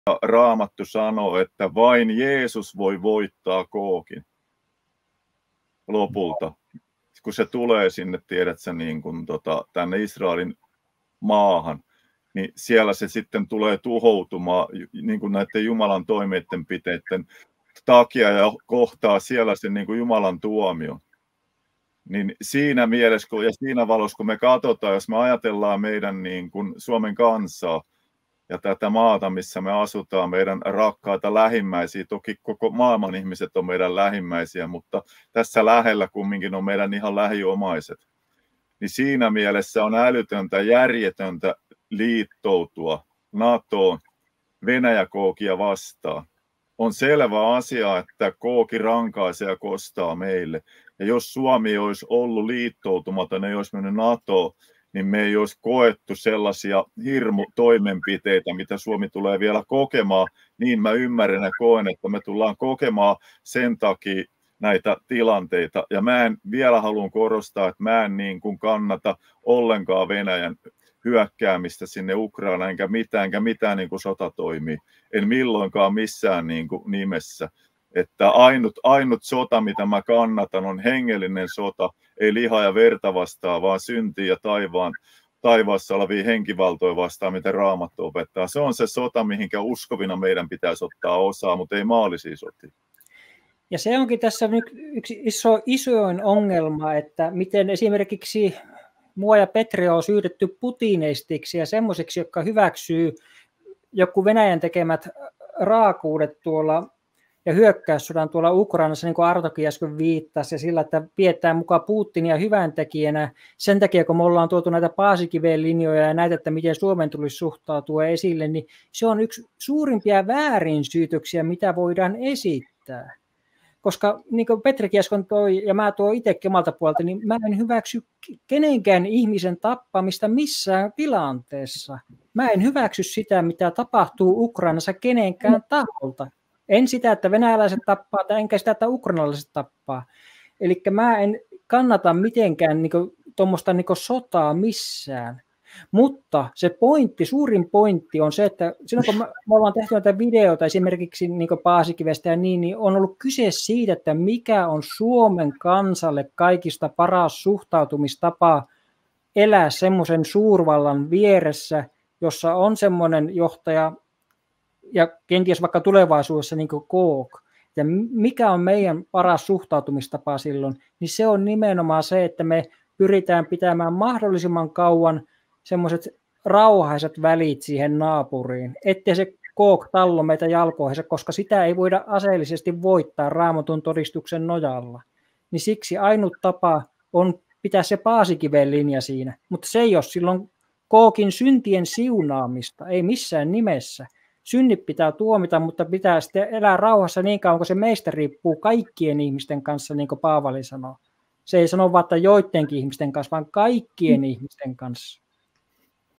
Raamatun ilmoituksen valossa: NATO-jäsenyyden vaarallisuus Suomelle
Kristittyjä, jotka ovat puolustaneet Paasikiven linjaa Suomen tulevaisuuden kannalta parhaana vaihtoehtona, on haukuttu Putinisteiksi. Tässä on lyhyt kiteytys siitä, miksi nämä syytökset ovat valheellisia ja irrationaalisia.